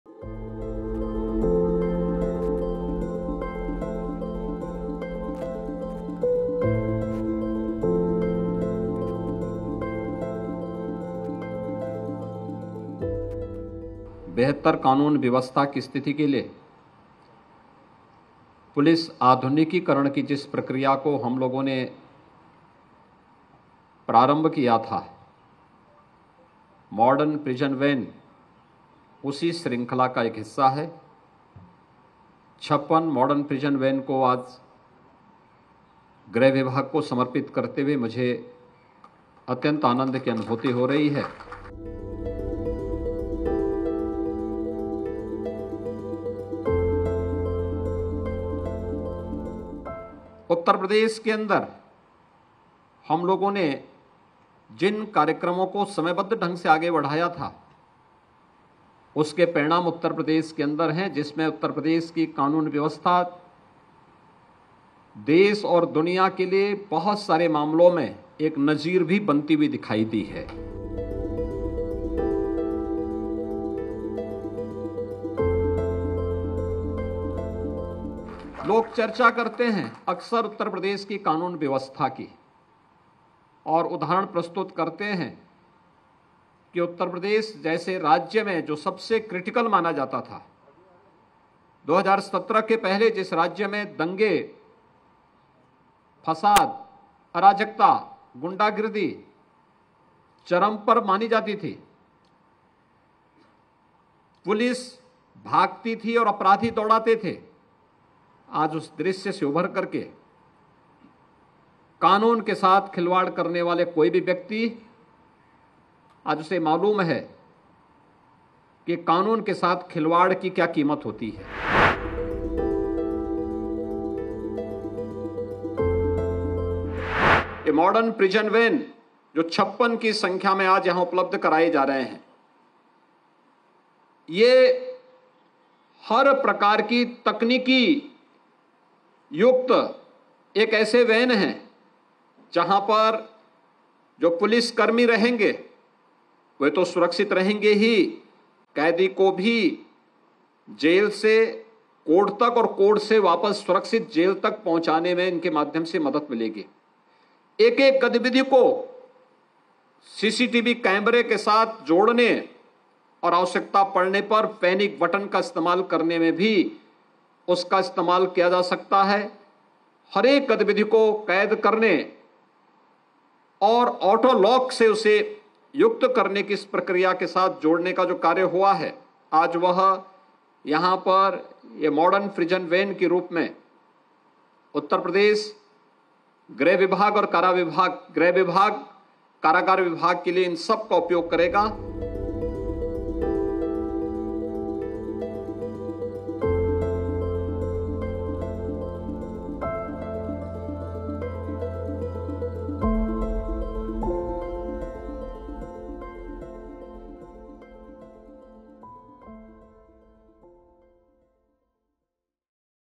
Uttar Pradesh में CM Yogi ने दिया कानून व्यवस्था को modern तोहफा
Uttar Pradesh के CM Yogi Adityanath कानून व्यवस्था पर हैं सख्त. UP की कानून व्यवस्था को CM Yogi ने बनाए रखने के लिए दिया तोहफा. 56 जिलों के लिए Yogi ने दी Modern Prison Van.